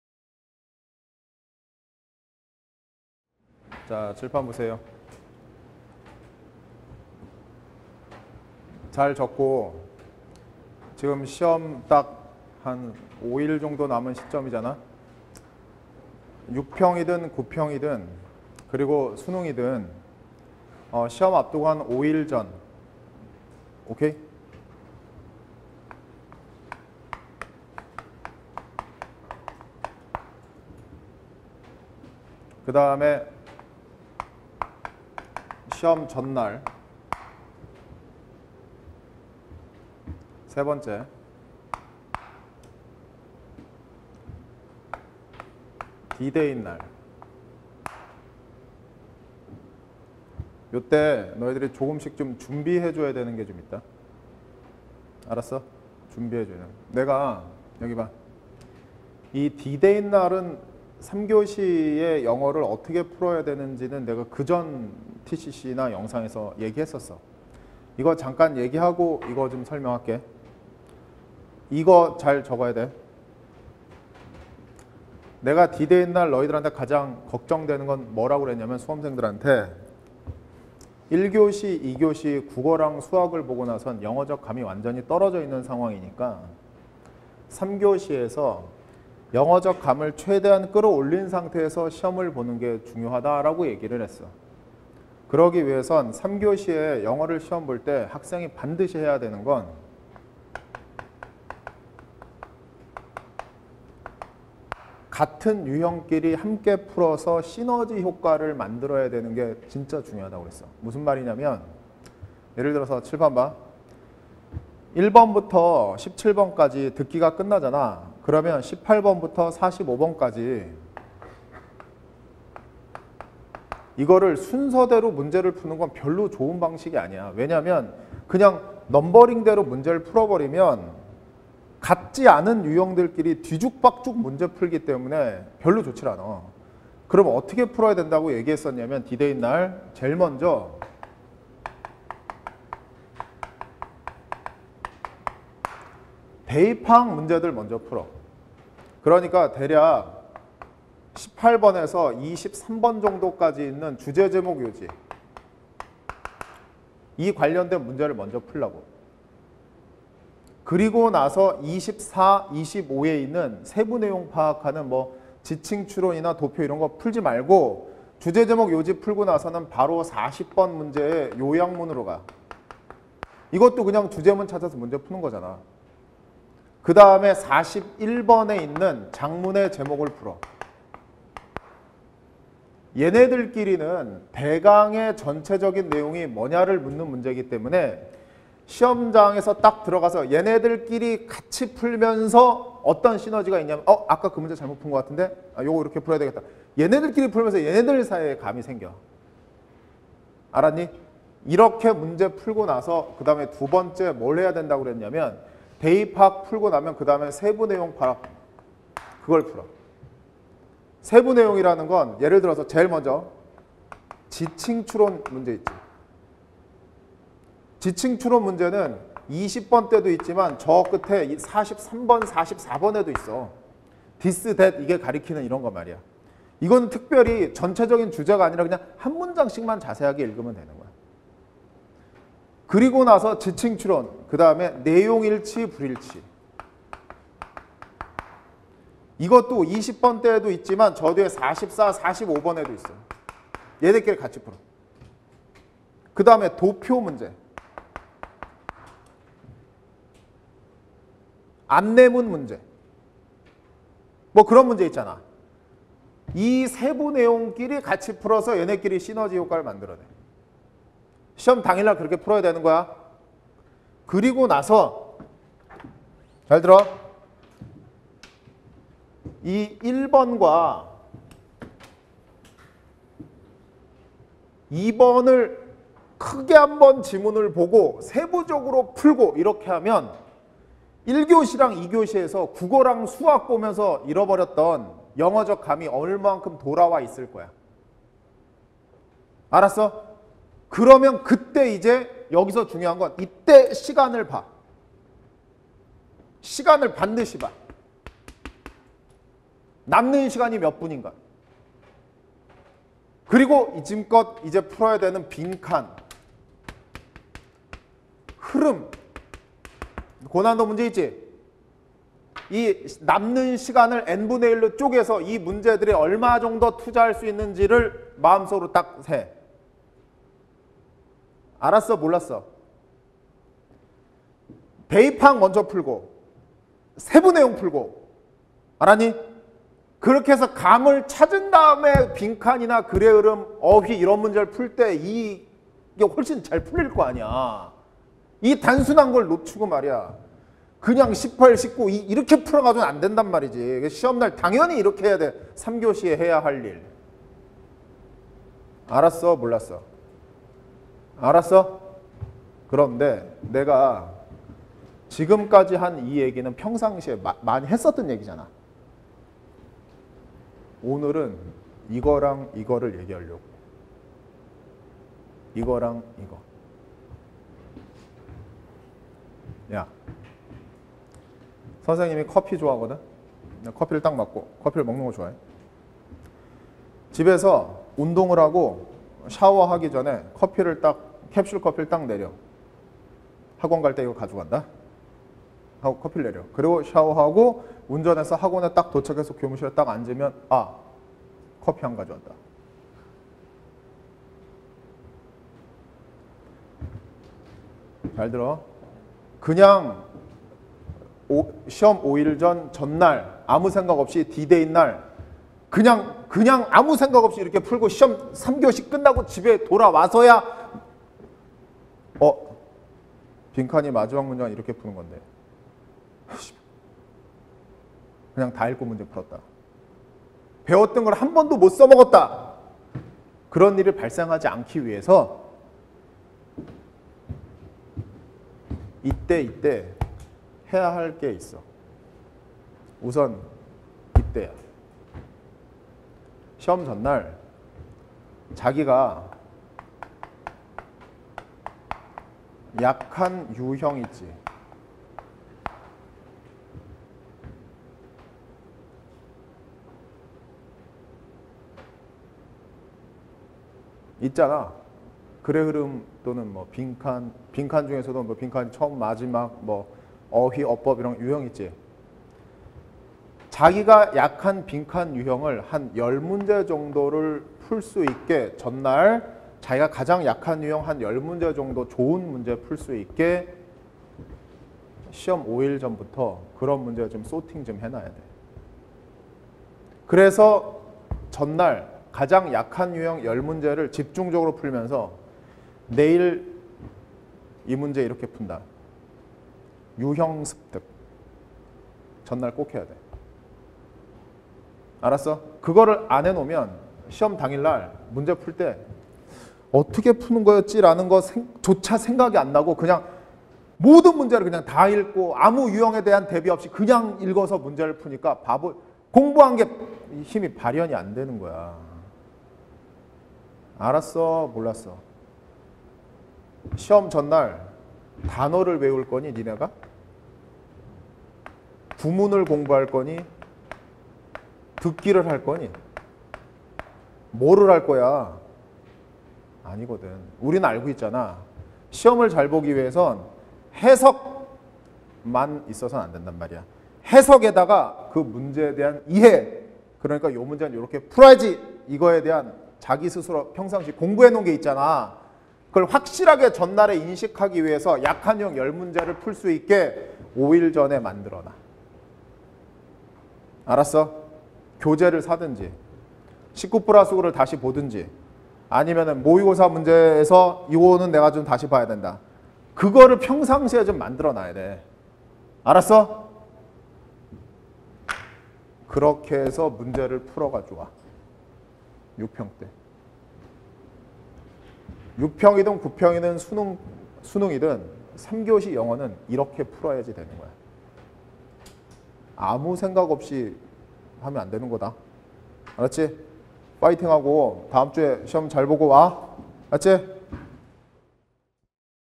[메가스터디] 영어 킹콩 쌤 - 6평을 바로 앞둔 지금, 이렇게 공부하세요!
1교시, 2교시를 보고 3교시 영어를 볼 때는
영어적 감각이 많이 떨어졌을 것입니다.

영어적 감각을 최대한으로 끌어올리고,
실력을 발휘할 수 있는 방법을 알려드립니다:)

3교시 영어를 풀 때는
비슷한 유형의 문제들을 모아서 풀며
시너지 효과를 낼 수 있도록 해야 합니다!

이를 위해서는 전날 약한 유형의 문제들을
풀어보는 것이 필요한데요,

6월 모평 3교시 영어에서
지금까지 공부한 실력을 발휘할 수 있는 방법,

킹콩 선생님의 강의가 궁금하다면?